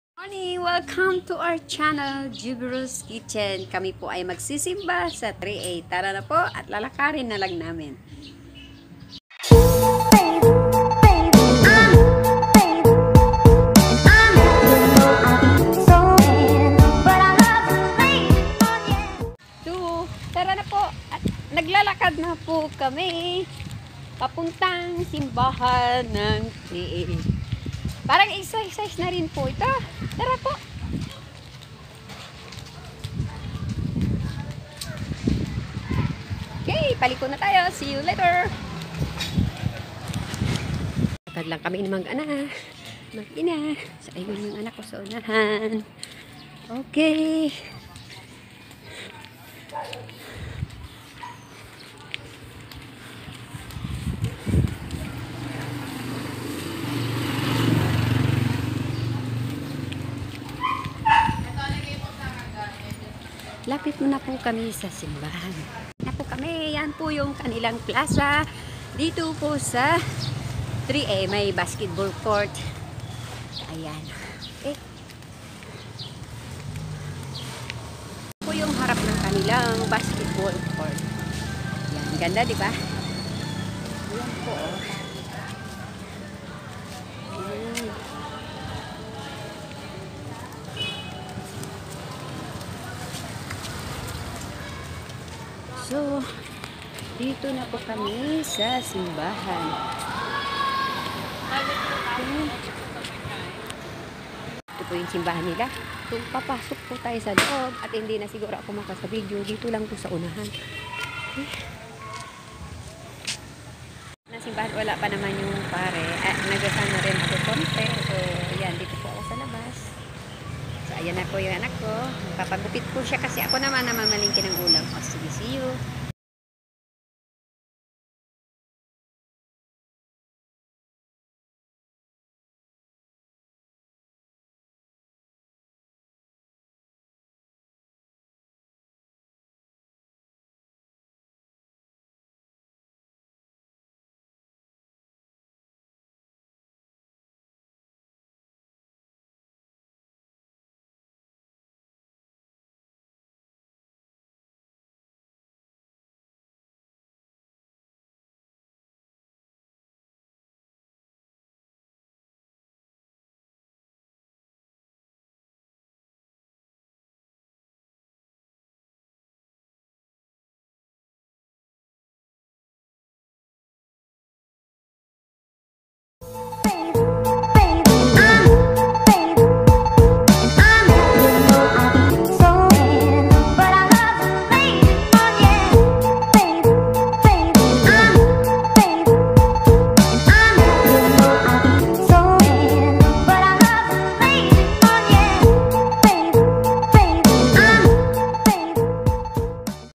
SUNDAY MASS TNR 3A - JUVIE ROSE KITCHEN
Gd am/pm mga guys nagsimba kami sa TNR 3A at nag kuha rin ako ng mga brautiful views doon. Pa watch po together wuth the ads salamat po.

Juvie Rose Kitchen